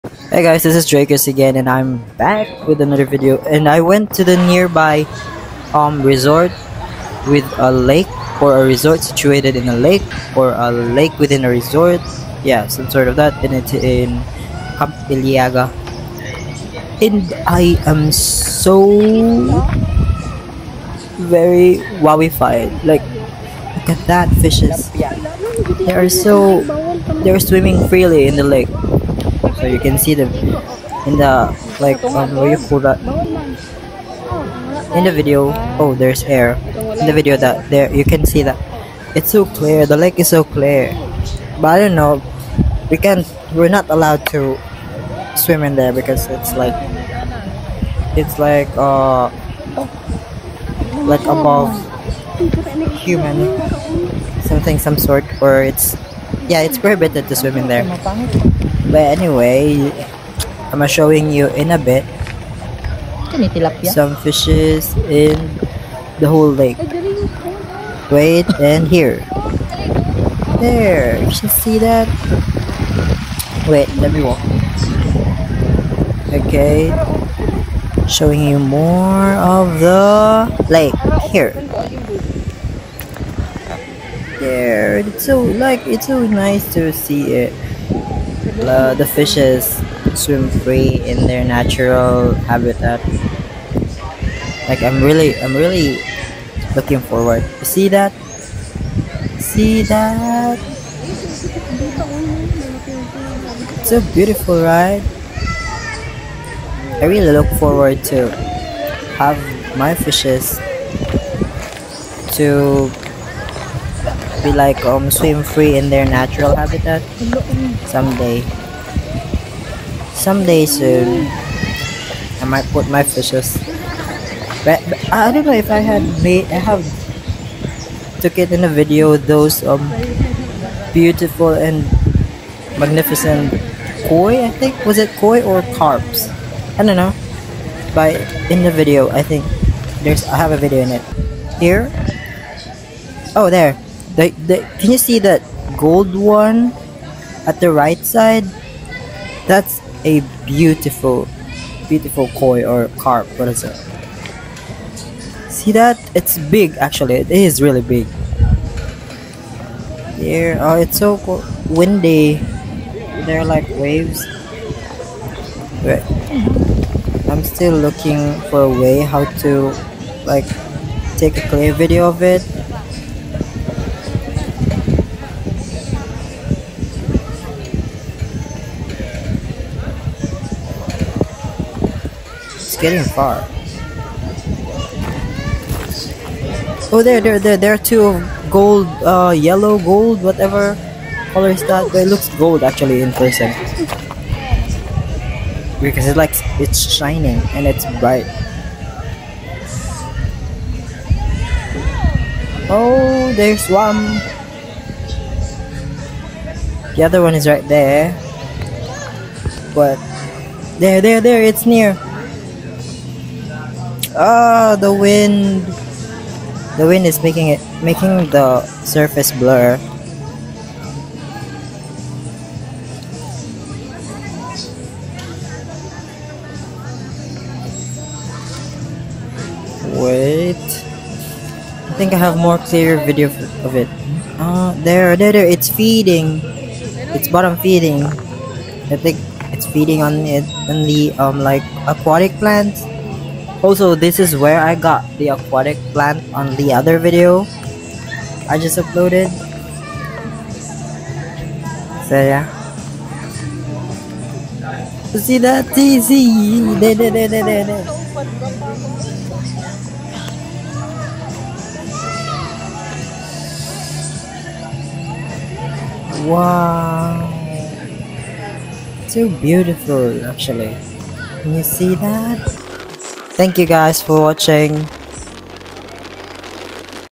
[LAKE] went to lake, saw massive SHINY GOLDEN CARP or KOI?
I would love to have my goldfish swim freely like the fishes I saw in the lake. there are like different species of fish in the lake (from freshwater shrimp, gurami, to golden carp or koi) swimming freely in the lake, and I am really amazed how nice it is to see them in the nature.